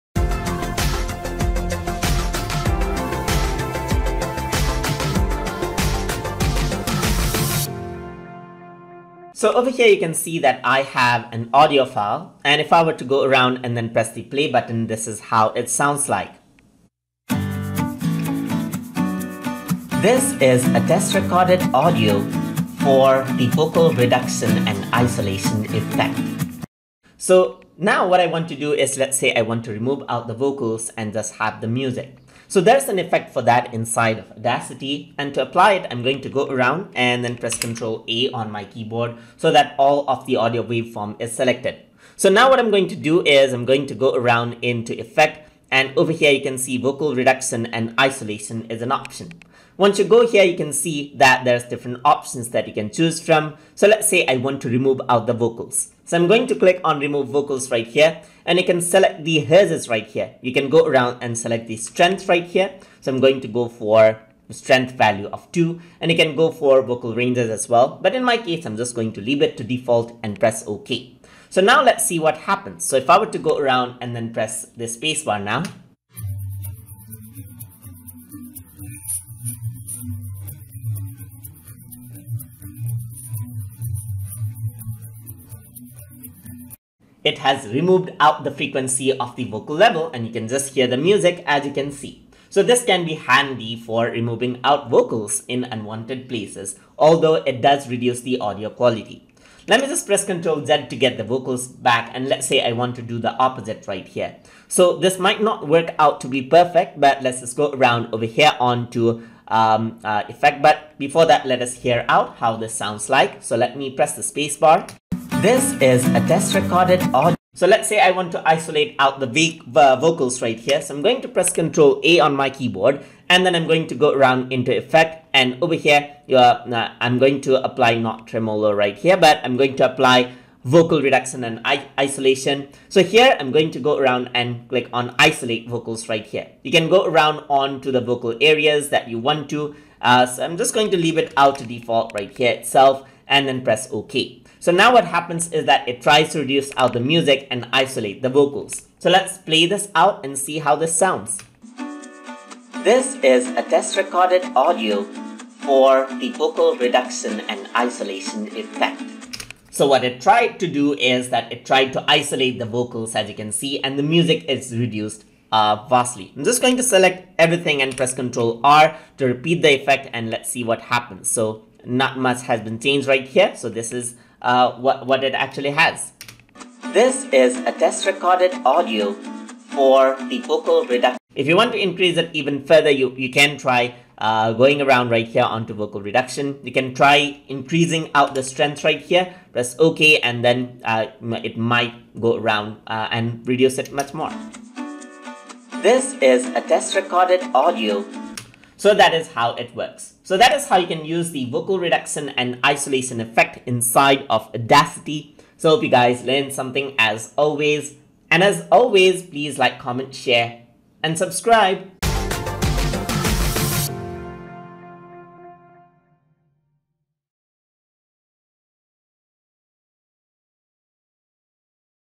Audacity Tutorial - Lesson 78 - Vocal Reduction and Isolation
In this tutorial, we will be discussing about Vocal Reduction and Isolation in Audacity